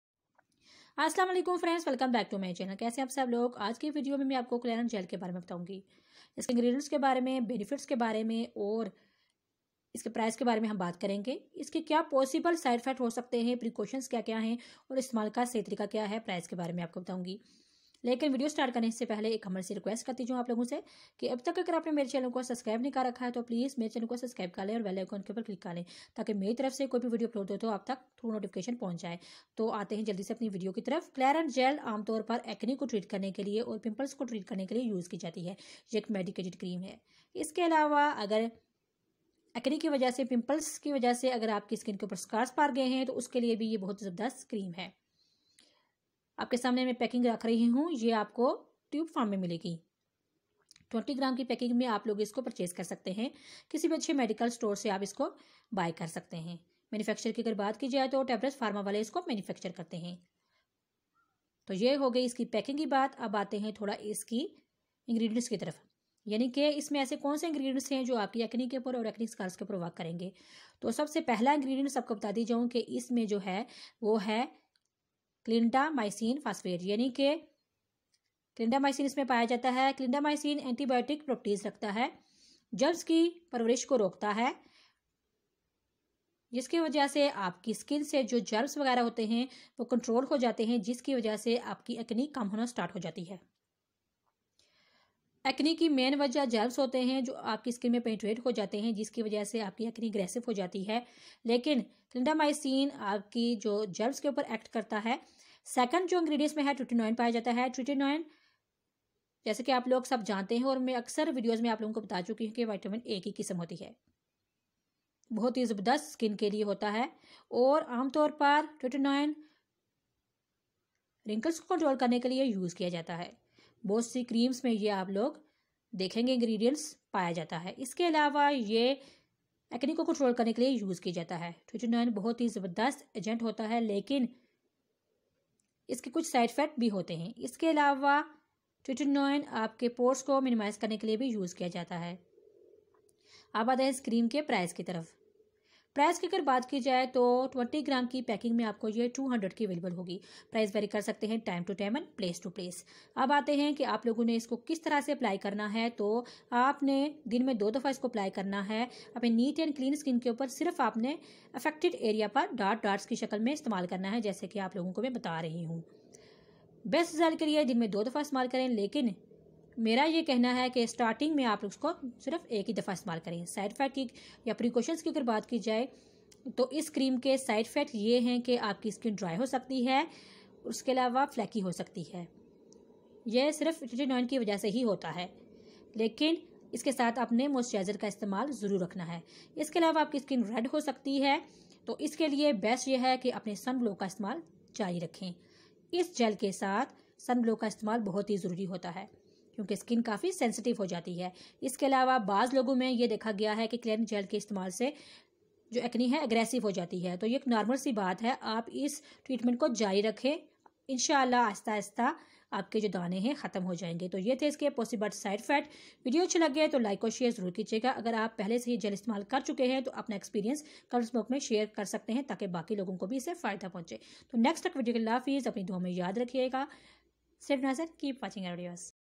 How to treat acne and acne scars with cleret gel | Best medicated treatment for acne and acne scars
How to treat acne a d acne scars |  Best medicated treatment for acne and acne scars